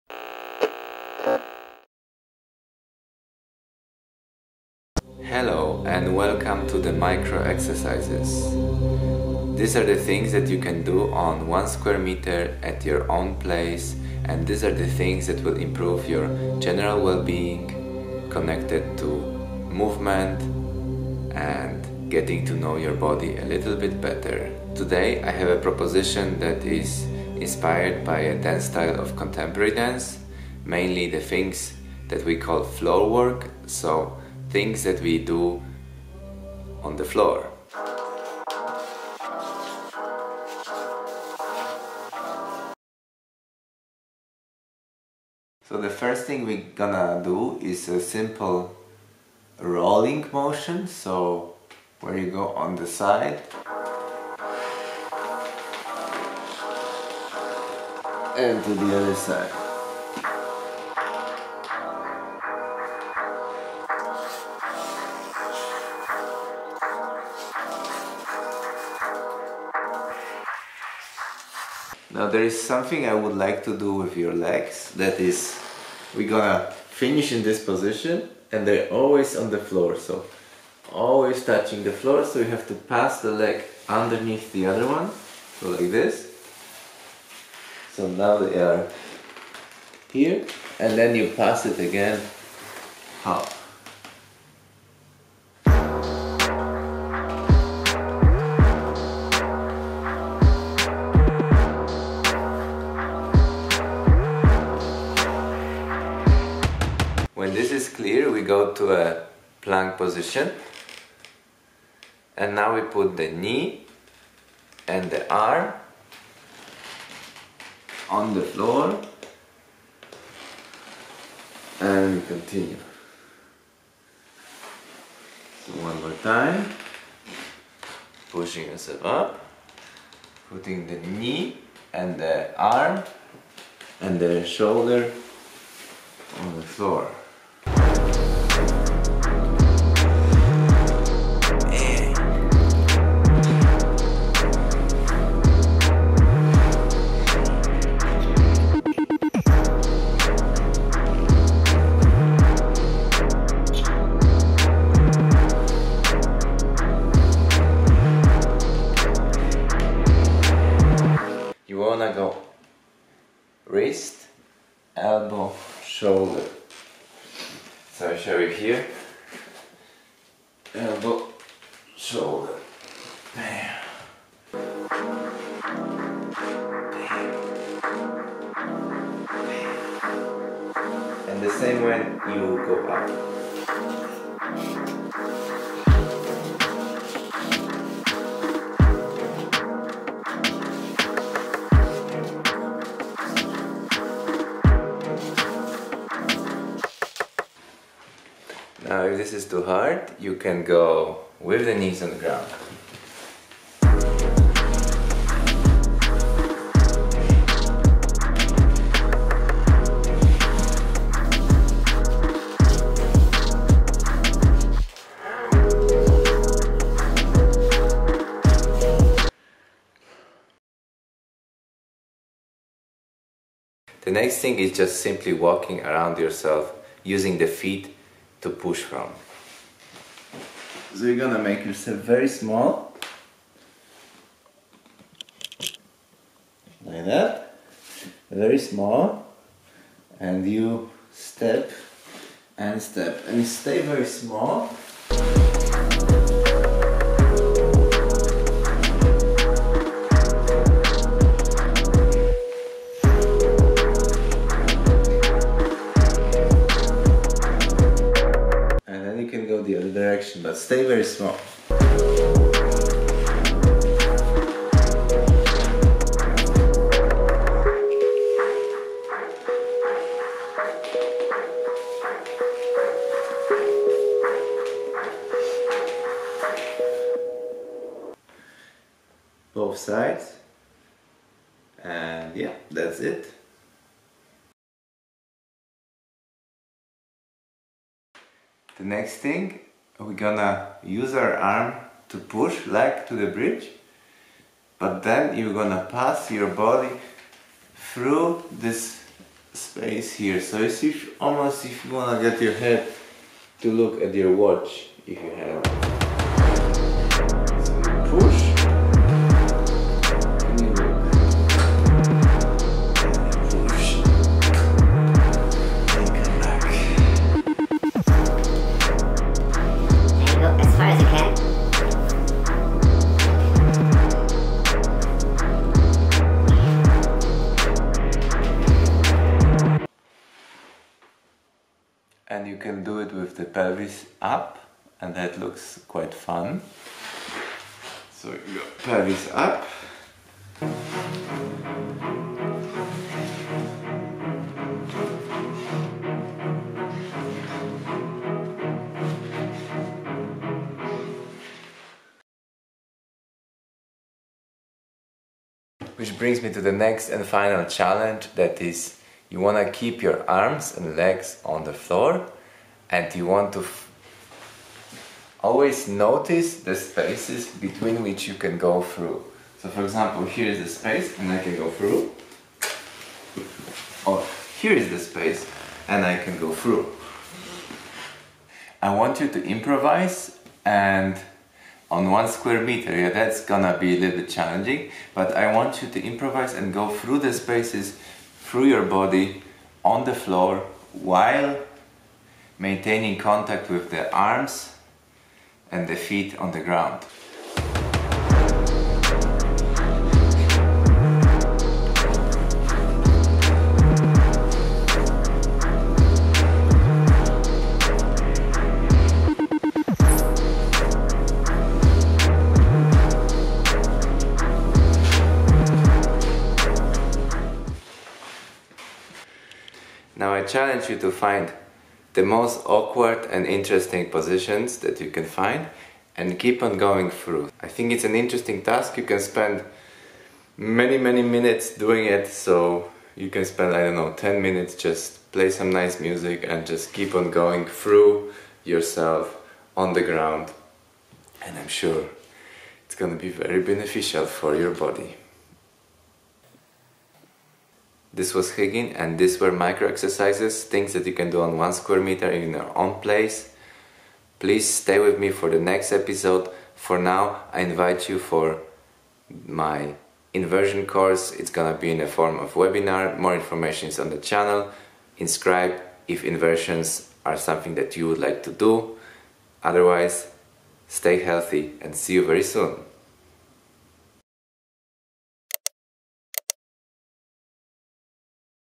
Contemporary dance inspired body mobility workout - One square meter (during quarantine) VOL.5
This time let's get inspired by the contemporary dance floorwork!
I am drawing my experience from my long experience in practicing and teaching dance (contemporary, breakdance, ballet). Share your experiece in the comments.

Pay attention to your feet! Enjoy!